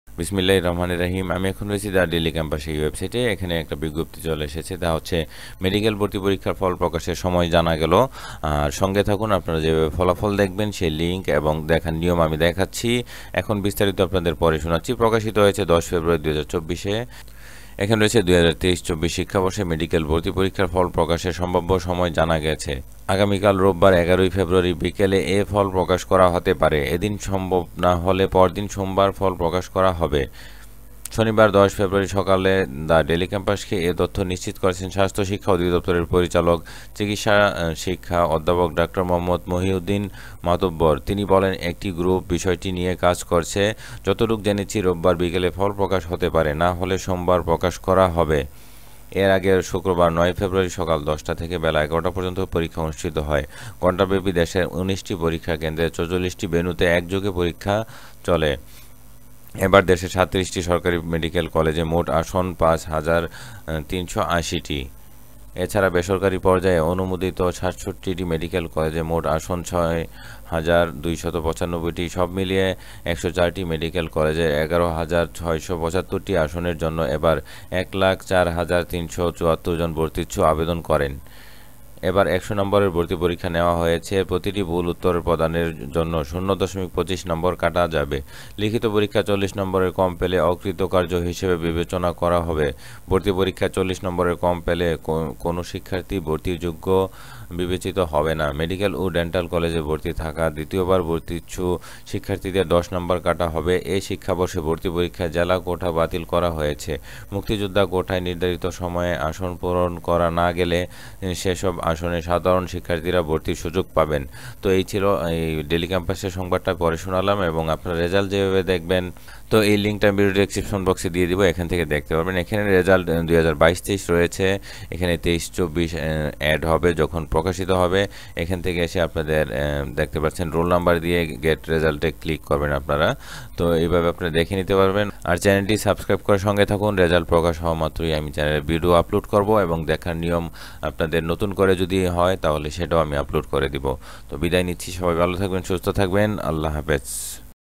মেডিকেল ভর্তি পরীক্ষার ফল প্রকাশের তারিখ-২০২৪। MBBS Admission test Exam Result-2024 Publish Date
মেডিকেল ভর্তি পরীক্ষার ফল প্রকাশের তারিখ-২০২৪।  MBBS Admission test Result 2024 Publish Date
২০২৩-২৪ শিক্ষাবর্ষে মেডিকেল ভর্তি পরীক্ষার ফল প্রকাশের সম্ভাব্য সময় জানা গেছে। আগামীকাল রোববার (১১ ফেব্রুয়ারি) বিকেলে এ ফল প্রকাশ করা হতে পারে। এদিন সম্ভব না হলে পরদিন সোমবার ফল প্রকাশ করা হবে।
শনিবার (১০ ফেব্রুয়ারি) সকালে দ্য ডেইলি ক্যাম্পাসকে এ তথ্য নিশ্চিত করেছেন স্বাস্থ্য শিক্ষা অধিদপ্তরের পরিচালক (চিকিৎসা শিক্ষা) অধ্যাপক ডা. মো. মহিউদ্দিন মাতুব্বর। তিনি বলেন, একটি গ্রুপ বিষয়টি নিয়ে কাজ করছে। যতটুকু জেনেছি, রোববার বিকেলে ফল প্রকাশ হতে পারে। না হলে সোমবার প্রকাশ করা হবে।

এর আগে শুক্রবার (৯ ফেব্রুয়ারি) সকাল ১০টা থেকে বেলা ১১টা পর্যন্ত পরীক্ষা অনুষ্ঠিত হয়। ঘণ্টাব্যাপী দেশের ১৯টি পরীক্ষা কেন্দ্রের ৪৪টি ভেন্যুতে একযোগে চলে পরীক্ষা। এবার দেশের ৩৭টি সরকারি মেডিকেল কলেজে মোট আসন ৫ হাজার ৩৮০টি। এ ছাড়া বেসরকারি পর্যায়ে অনুমোদিত ৬৭টি মেডিকেল কলেজে মোট আসন ৬ হাজার ২৯৫টি। সব মিলিয়ে ১০৪টি মেডিকেল কলেজের ১১ হাজার ৬৭৫টি আসনের জন্য এবার ১ লাখ ৪ হাজার ৩৭৪ জন ভর্তিচ্ছু আবেদন করেন।
Admission test has been held on 9 February 2024 for admission in MBBS course in government and private medical colleges in Bangladesh. Candidates who got 40 or more marks in the admission test have been included in the merit list. Candidates on the merit list are being informed of results through SMS. Students with got less than 40 marks are considered as unsuccessful.

Merit list has been prepared on the basis of total 300 marks. Of these, admission test has been taken for 100 marks. The remaining 200 marks are considered for SSC / equivalent and HSC equivalent examination results. SSC level is considered as 75 and HSC level is considered as 125.

There were a total of 100 MCQ questions for 100 marks in the admission test. The value of each question was 1. The duration of the test was 1 hour. The admission test had 20 marks in Physics, 25 marks in Chemistry, 30 marks in Biology, 15 marks in English, and 10 marks in general knowledge, history and liberation War.

0.25 marks are deducted for each wrong answer in the admission test. Students participating in the admission test for a second time are also deducted 5 marks. In the meantime, the merit list has been published by cutting the 10 marks of students who are admitted to any government medical college or dental college already.

How to know MBBS Result?
MBBS Result 2024 can be known from the Directorate General of Health Education DGME website dgme.gov.bd. Students who have passed will also be notified of the result via SMS. Follow the steps below to know MBBS Admission Result 2024.

Visit the results related website result.dghs.gov.bd.
Enter the MBBS Result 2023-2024 option.
Enter the Admission Test Roll number.
See the result by clicking on the result button.
Result by SMS
Students will be able to know the result through SMS. Students will be informed of the results by sending an SMS from the Directorate of Health Education. SMS will be sent to the mobile number provided by the student at the admission application time. Only successful students will be received SMS.